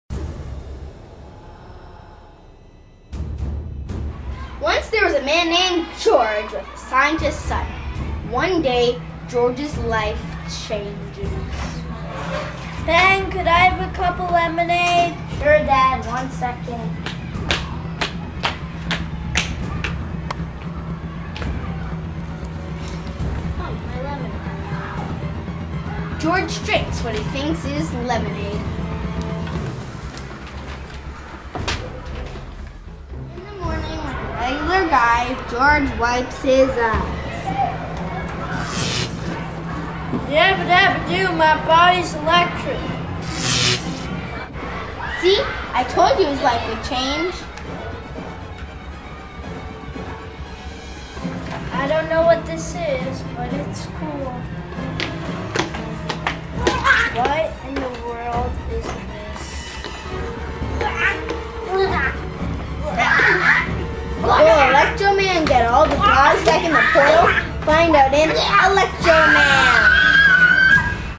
Reel Science Electric Man
Kids from 87 Afterschool program's Reel Science class created this trailer to demonstrate the Special Effect of "Slime"; polymers used for make-up of the blob creatures.

Elementary school students in Reel Science (the science of filmmaking and special effects) create an end of term project using the science behind some well know Hollywood special effects!